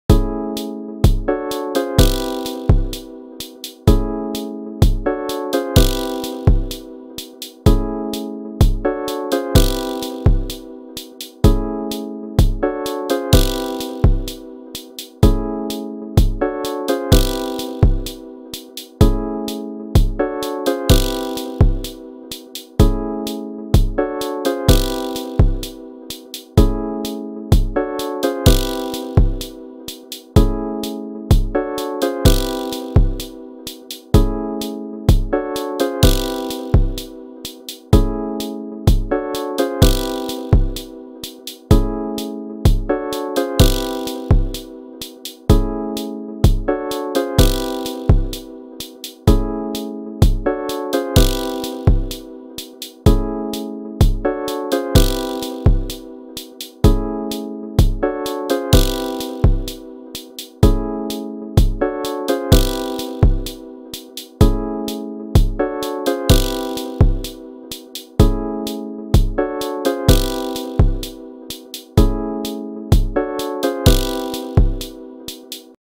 127 Bpm Hip Hop Beat Loop
127 bpm hip hop beat loop. Downtempo instrumental chill beat. Music drum loop.